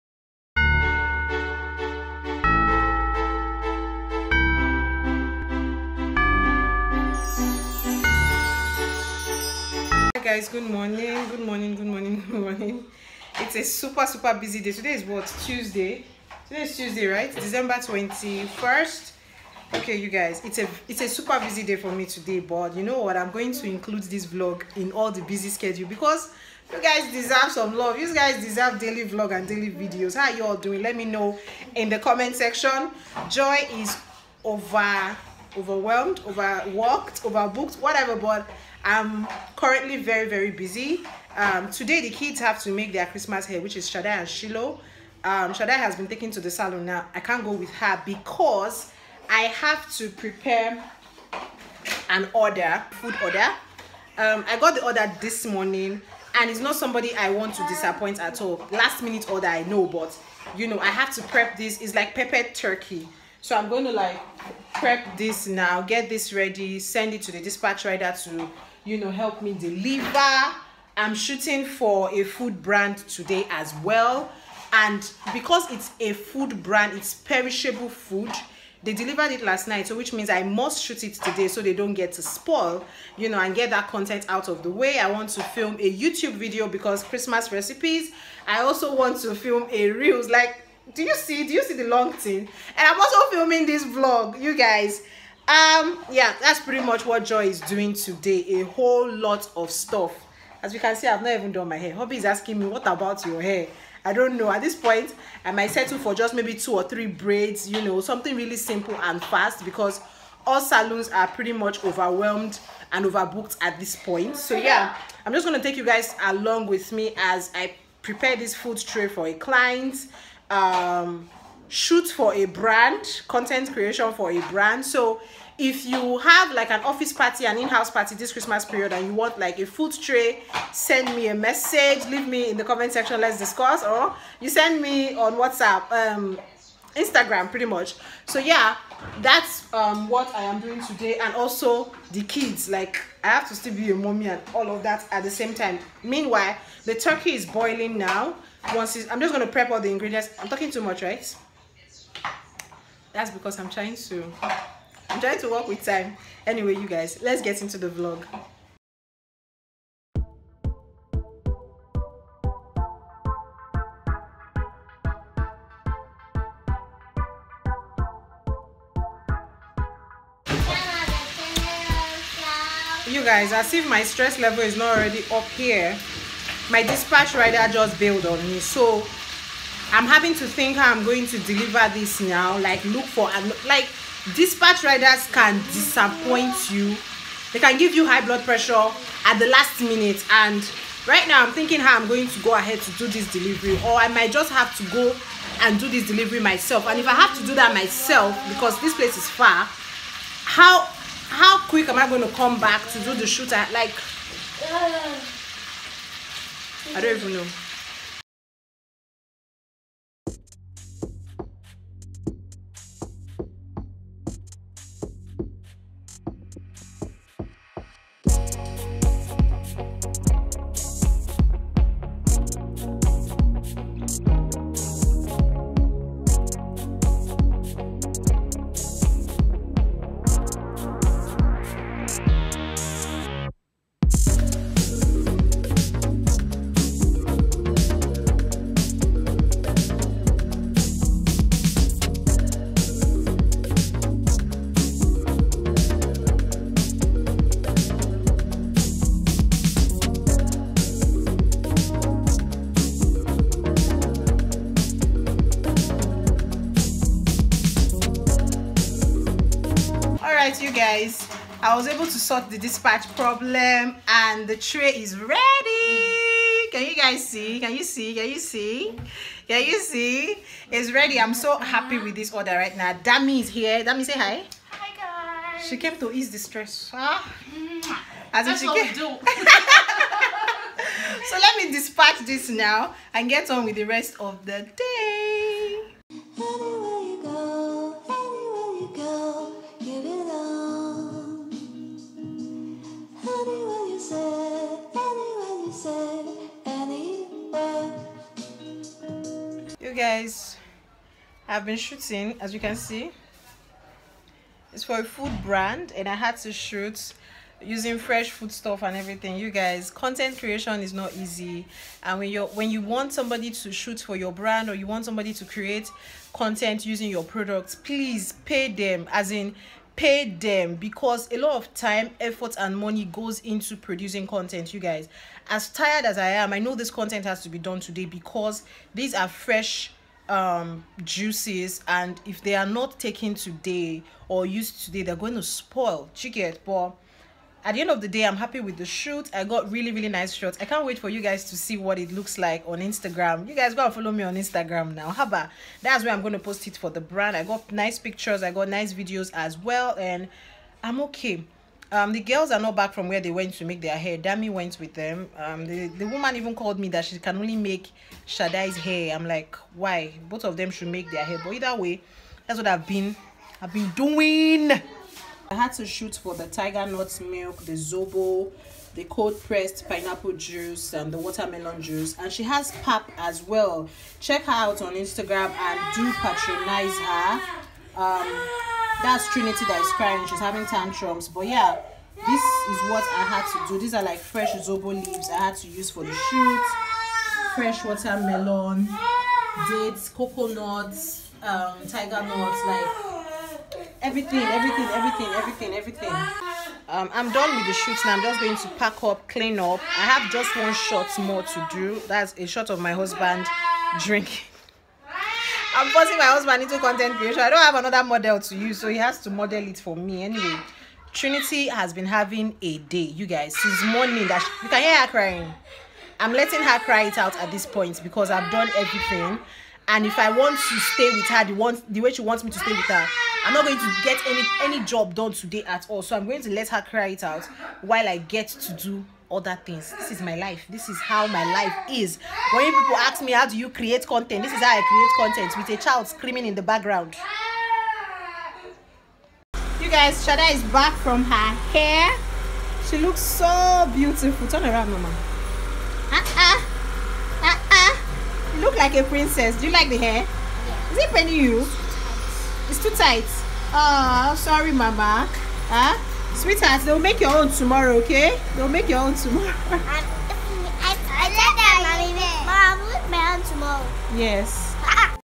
Food Vending + Creating Content For A Brand + Balancing Motherhood+ A lot of Stress| Vlogmas day 21
Today’s vlogmas video I show you how I create content for a brand, cook and pack meals for clients as well as be a mum in the midst of all these, mum life isn’t easy at all.